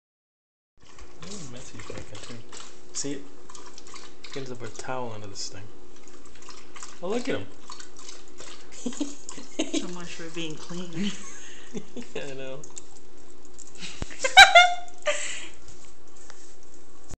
Beagle drinking water
Our Beagle, Scooby, got a new water fountain/filter! This is the live footage of his first sips of water from it!